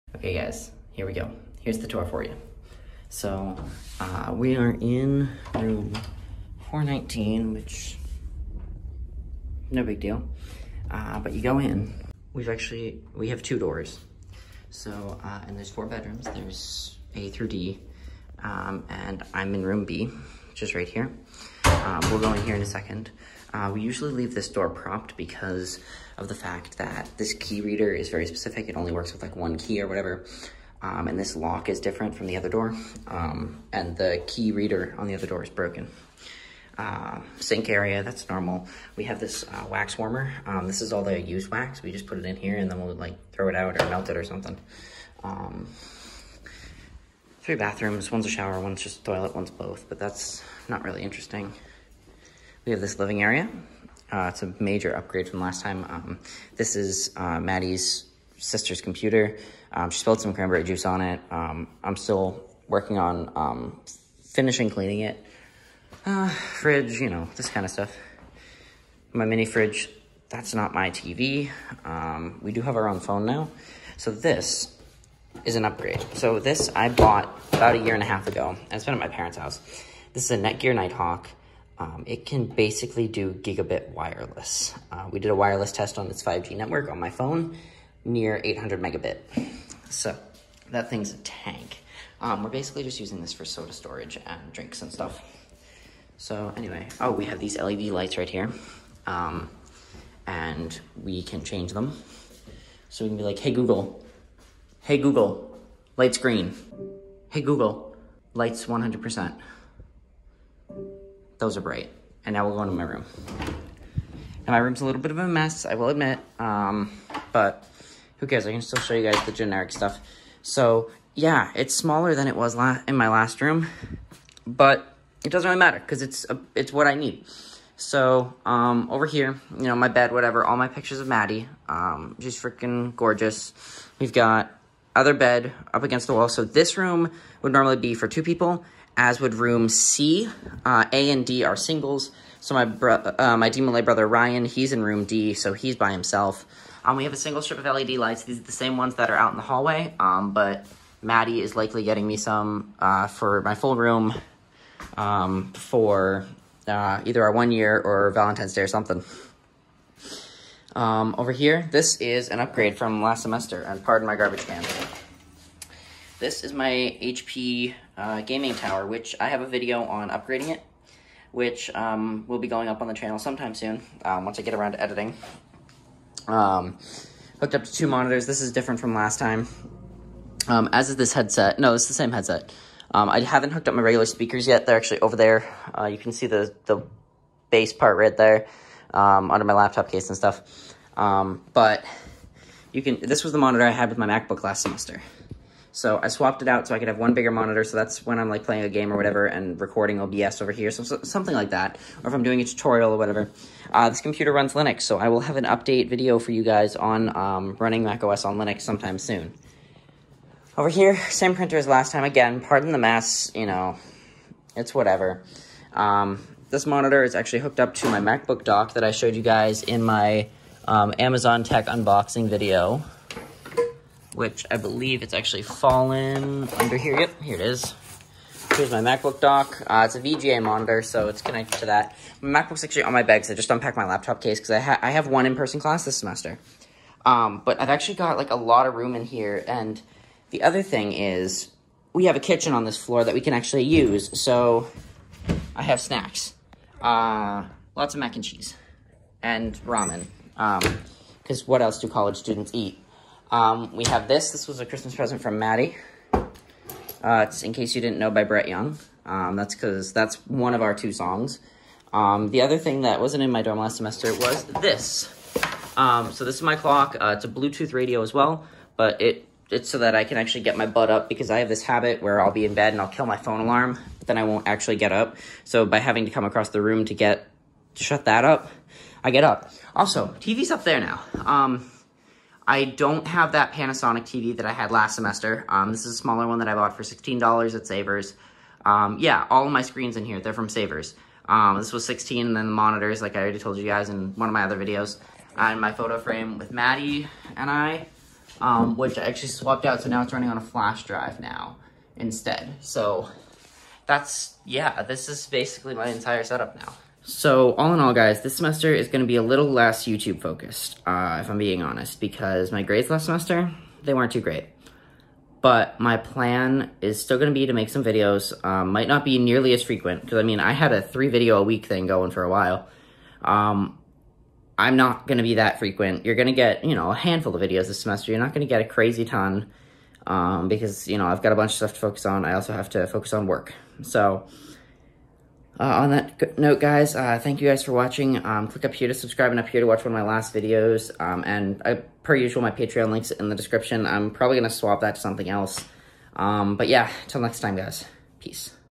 Spring 2021 Dorm Tour!!!!
Hey everyone! This video is a tour of my dorm setup for the Spring 2021 semester. I hope you all enjoy!